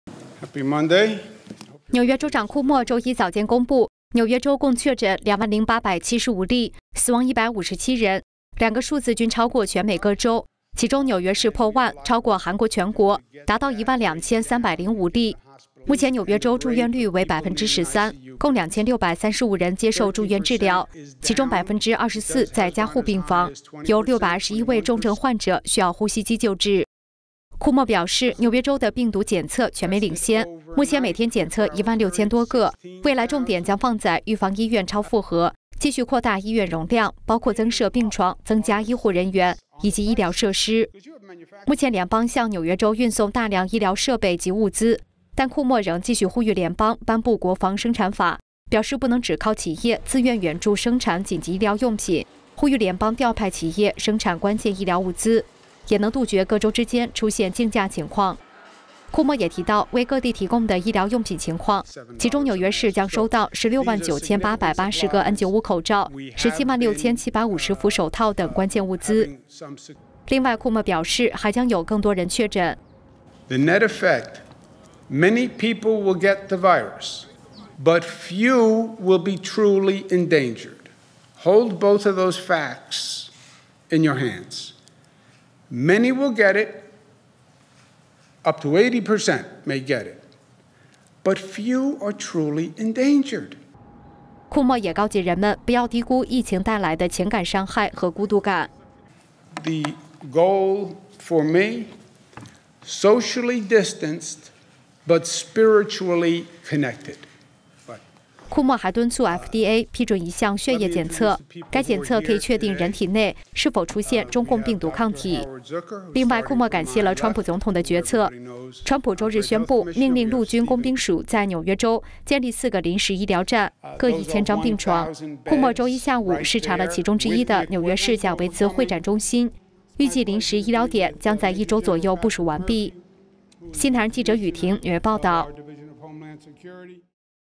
疫情严峻 纽约州长：最多80%确诊
纽约州长库默（Andrew Cuomo ），周一（3月23日）早间公布，纽约州共确诊20,875例，死亡157人，两个数字均超过全美各州，其中纽约市破万，超越韩国全国，达到12,305例。